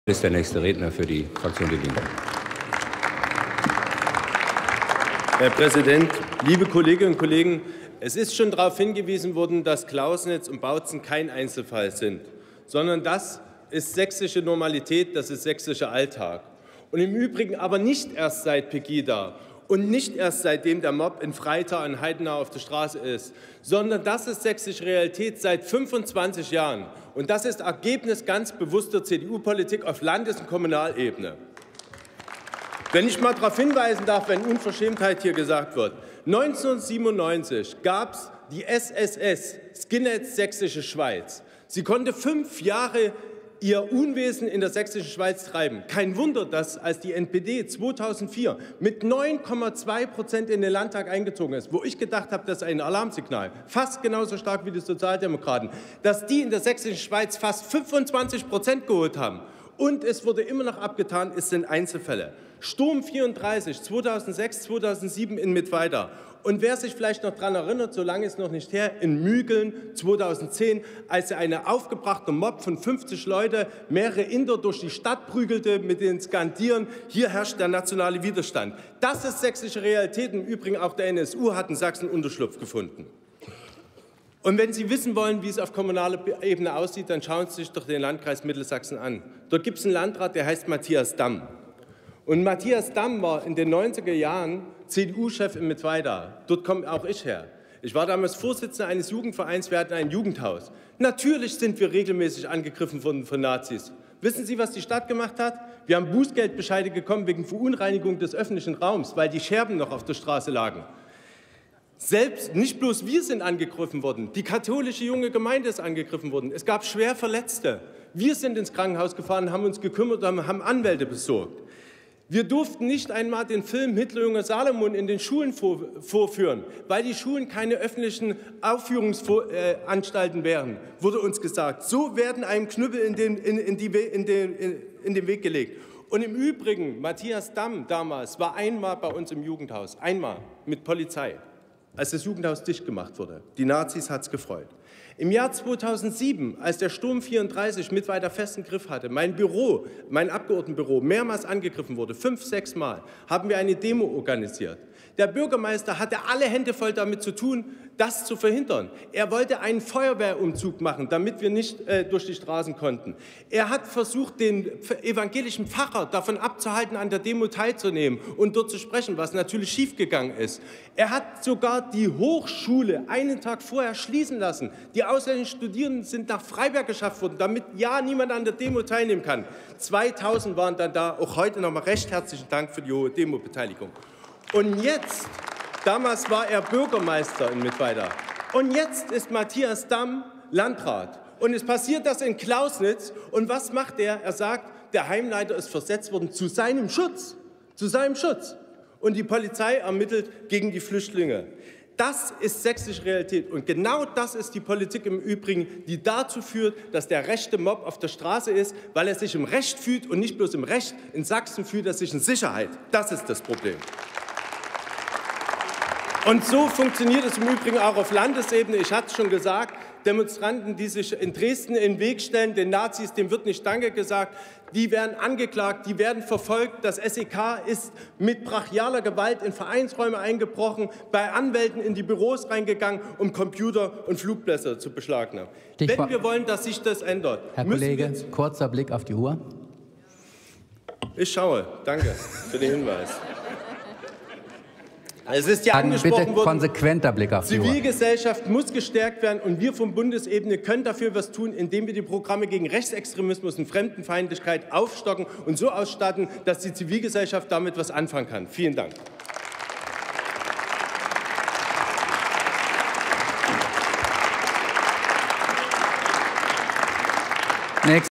Michael Leutert: Vereinbarte Debatte zu den Ereignissen von Clausnitz und… [Bundestag 24.02.2016]
Vereinbarte Debatte
zu den Ereignissen von Clausnitz und Bautzen

18. Deutscher Bundestag
157. Plenarsitzung
Tagesordnungspunkt: Vereinbarte Debatte zu den Ereignissen von Clausnitz und Bautzen
Redner: Leutert, Michael, Die Linke
Partei: Die Linke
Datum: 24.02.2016
Plenarprotokoll:
UVID:6590654